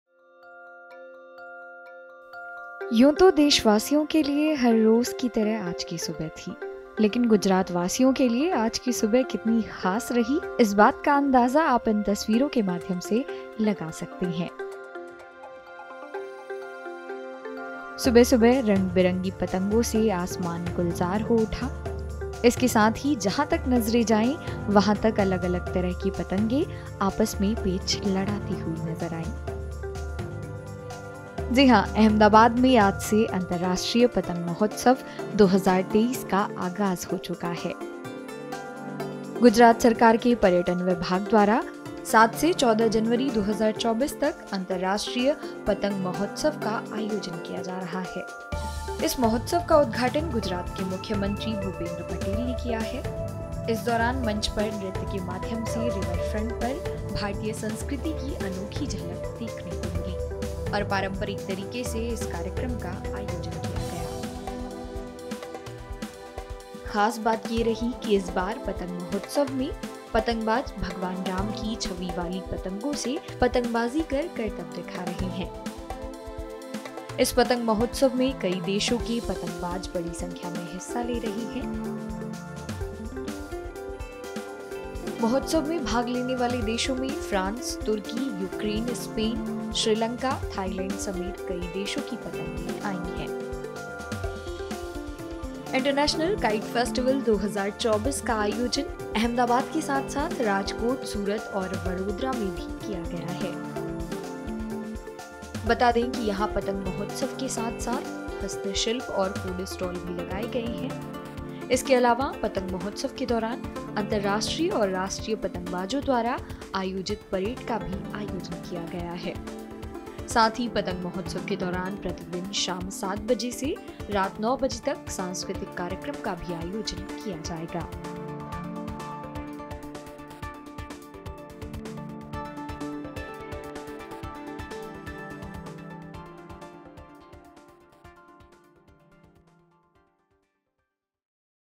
Ahmedabad में International Kite Festival का आगाज, आसमान में दिखी भगवान राम की छवी वाली पतंगें
यूं तो देशवासियों के लिए हर रोज की तरह आज की सुबह थी, लेकिन गुजरातवासियों के लिए आज की सुबह कितनी ख़ास रही इस बात का अंदाजा आप इन तस्वीरों के माध्यम से लगा सकते हैं, सुबह-सुबह रंग बिरंगी पतंगों से आसमान गुलजार हो उठा इसके साथ ही जहां तक नजरें जाए वहां तक अलग अलग तरह की पतंगे आपस में पेंच लड़ाती हुई नज़र आईं । जी हां अहमदाबाद में आज से अंतर्राष्ट्रीय पतंग महोत्सव 2024 का आगाज हो चुका है। गुजरात सरकार के पर्यटन विभाग द्वारा 7 से 14 जनवरी, 2024 तक अंतर्राष्ट्रीय पतंग महोत्सव का आयोजन किया जा रहा है।